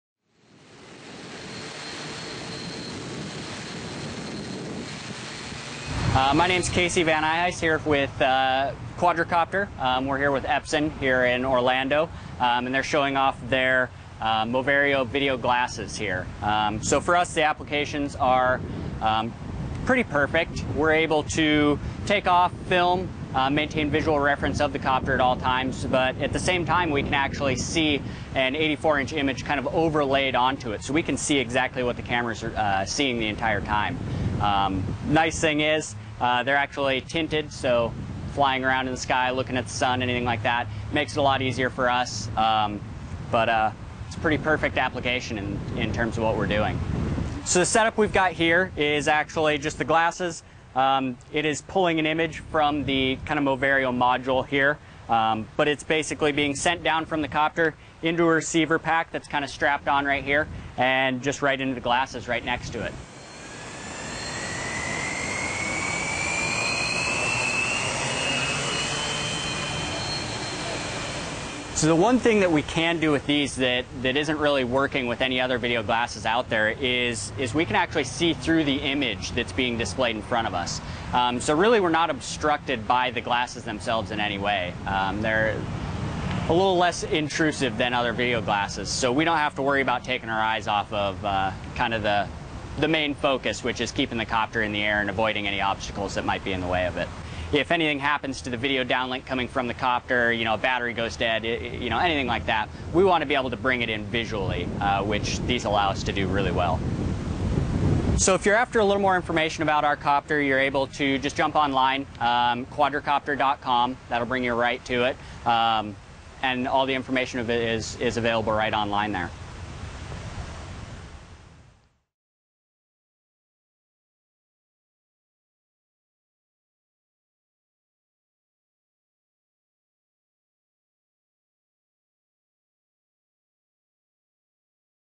First Person View - Aerial videography using Epson Moverio glasses
Aerial photographer Casey Van Nyhuis demonstrates how he uses the see-through nature of Epson Moverio BT-100 glasses to achieve both a first person camera view while maintaining safety through constant visual contact with the quadrocopter.  For more information please visit: